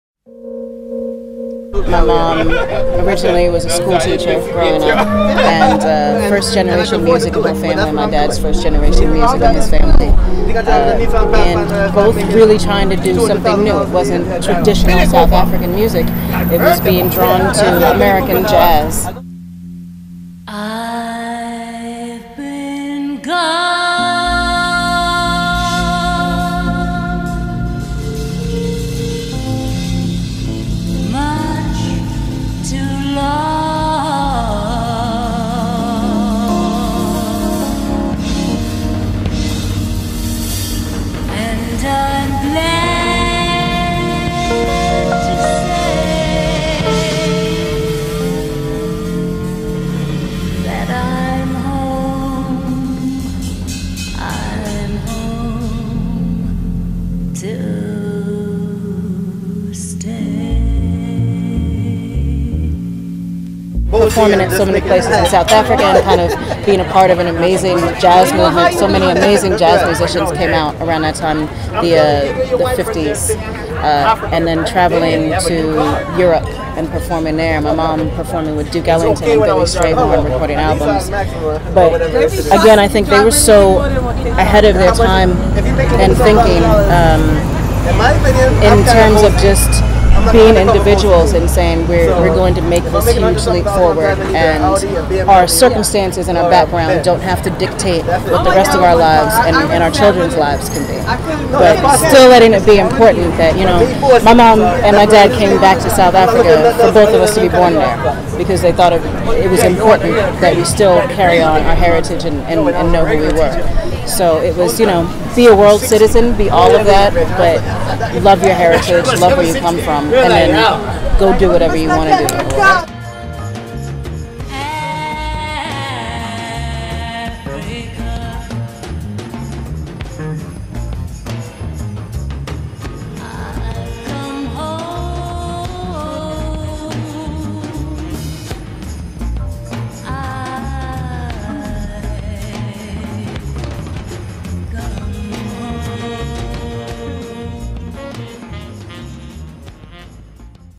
South African Songbird, Sathima Bea Benjamin Passes Away At 76
Twitter.com/RevoltTV
Instagram.com/RevoltTV
RevoltTV.Tumblr.com
Revolt.TV